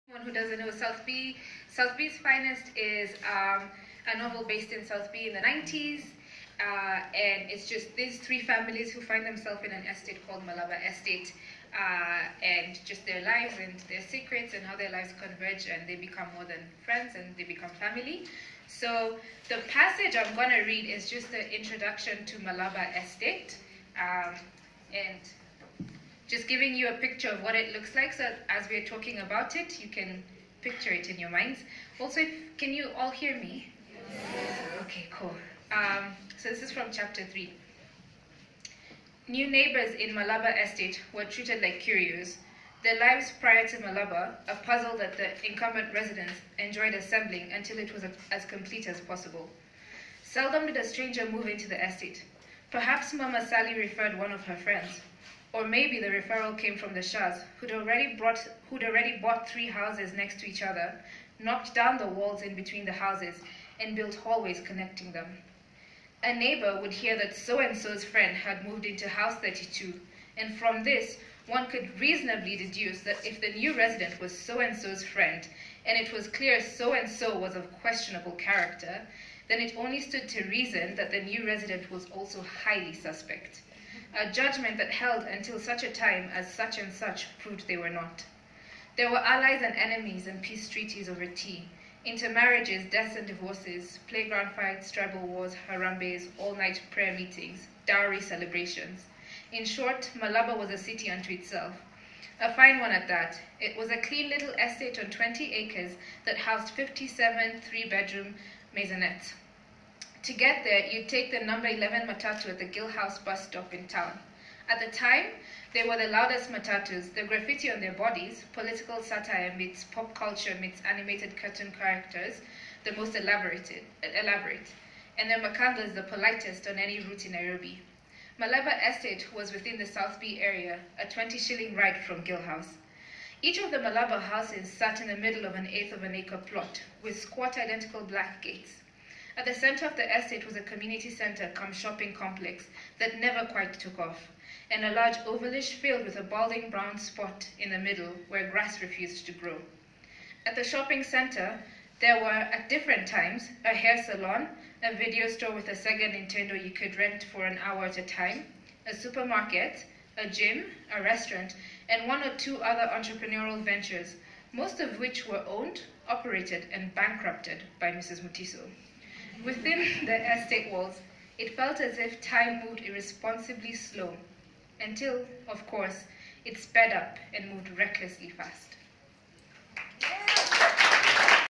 Makena Maganjo reads from her debut novel "South B's Finest" at the launch in Nairobi.
Makena Maganjo reads from her debut novel "South B's Finest" when it launched at the Goethe-Institut, Nairobi on March 5, 2020.